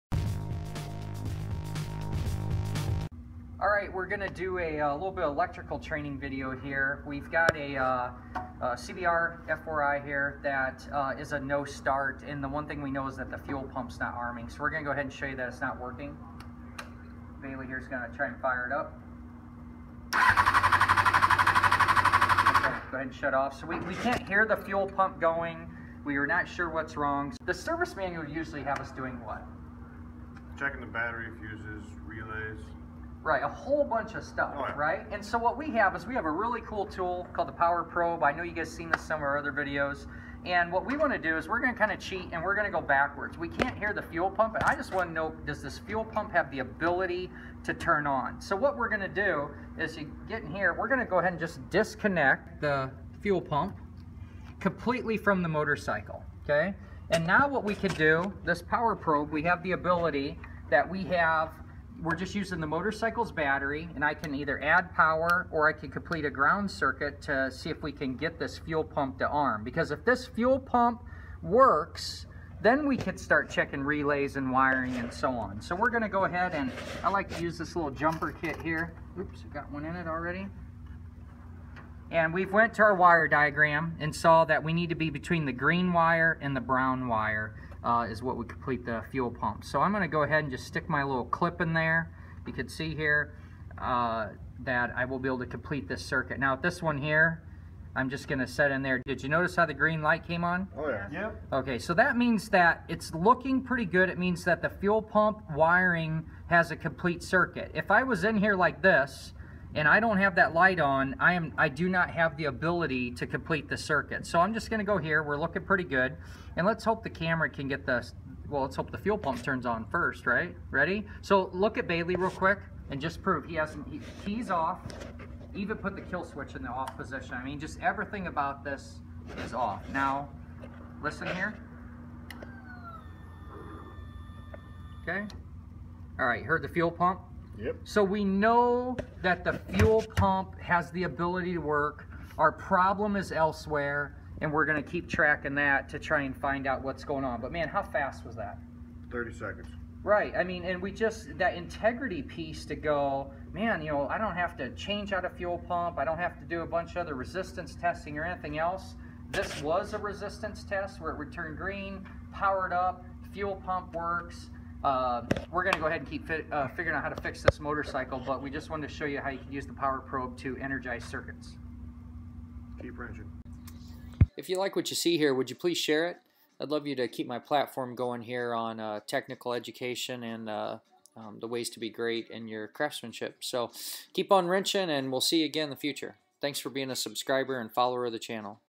Power Probe Training: How to prove a fuel pump will turn on when powered. How to bypass the wiring!
In our Power Probe Training class, I show how to bypass the motorcycles electrical system and power the fuel pump independently! This tool speeds up electrical testing immensely.

PowerProbe 3: (Tool no case)
PP3 with case
New PP4: (does volts, amps and ohms!)

Keep Wrenching,
Shane Conley
Instructor/Motivator/Motorcyclist